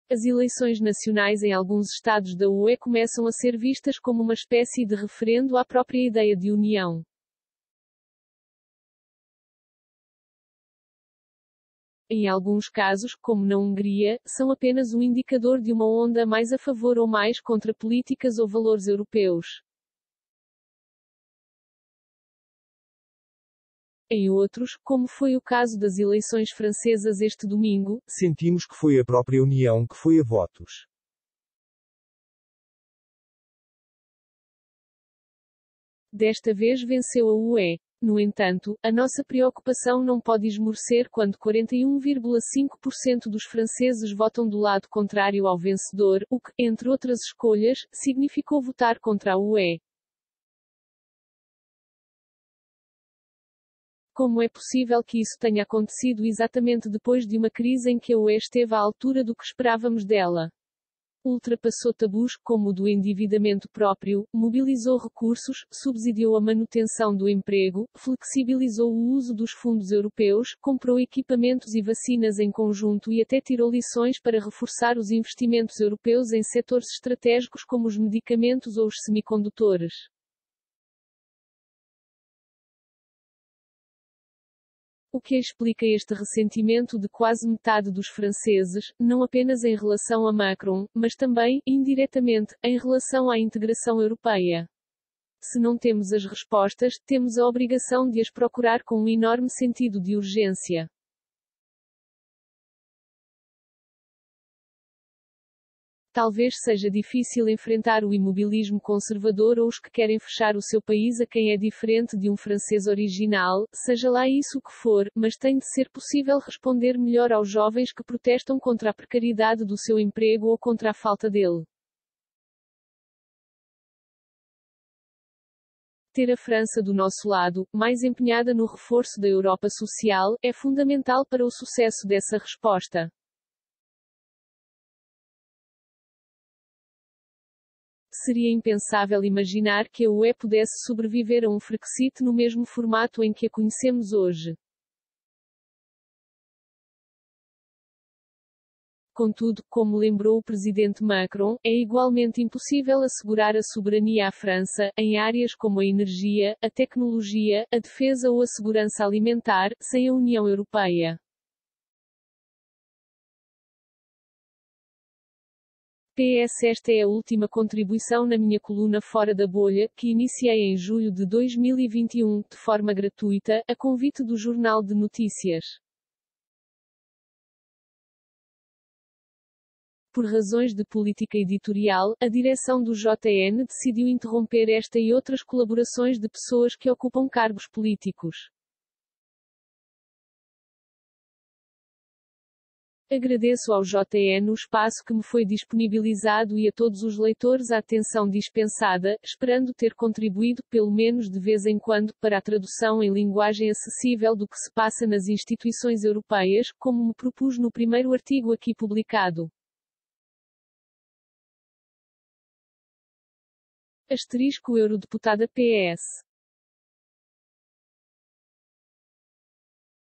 A União Europeia foi a votos
As eleições nacionais em alguns estados da UE começam a ser vistas como uma espécie de referendo à própria ideia de União. Em alguns casos, como na Hungria, são apenas um indicador de uma onda mais a favor ou mais contra políticas ou valores europeus. Em outros, como foi o caso das eleições francesas este domingo, sentimos que foi a própria União que foi a votos. Desta vez venceu a UE. No entanto, a nossa preocupação não pode esmorecer quando 41,5% dos franceses votam do lado contrário ao vencedor, o que, entre outras escolhas, significou votar contra a UE. Como é possível que isso tenha acontecido exatamente depois de uma crise em que a UE esteve à altura do que esperávamos dela? Ultrapassou tabus, como o do endividamento próprio, mobilizou recursos, subsidiou a manutenção do emprego, flexibilizou o uso dos fundos europeus, comprou equipamentos e vacinas em conjunto e até tirou lições para reforçar os investimentos europeus em setores estratégicos como os medicamentos ou os semicondutores. O que explica este ressentimento de quase metade dos franceses, não apenas em relação a Macron, mas também, indiretamente, em relação à integração europeia? Se não temos as respostas, temos a obrigação de as procurar com um enorme sentido de urgência. Talvez seja difícil enfrentar o imobilismo conservador ou os que querem fechar o seu país a quem é diferente de um francês original, seja lá isso o que for, mas tem de ser possível responder melhor aos jovens que protestam contra a precariedade do seu emprego ou contra a falta dele. Ter a França do nosso lado, mais empenhada no reforço da Europa social, é fundamental para o sucesso dessa resposta. Seria impensável imaginar que a UE pudesse sobreviver a um "Frexit" no mesmo formato em que a conhecemos hoje. Contudo, como lembrou o presidente Macron, é igualmente impossível assegurar a soberania a França, em áreas como a energia, a tecnologia, a defesa ou a segurança alimentar, sem a União Europeia. PS Esta é a última contribuição na minha coluna "Fora da Bolha", que iniciei em julho de 2021, de forma gratuita, a convite do "Jornal de Notícias". Por razões de politica editorial, a direção do JN decidiu interromper esta e outras colaborações de pessoas que ocupam cargos políticos. Agradeço ao JN o espaço que me foi disponibilizado e a todos os leitores a atenção dispensada, esperando ter contribuído, pelo menos de vez em quando, para a tradução em linguagem acessível do que se passa nas instituições europeias, como me propus no primeiro artigo aqui publicado. *Eurodeputada PS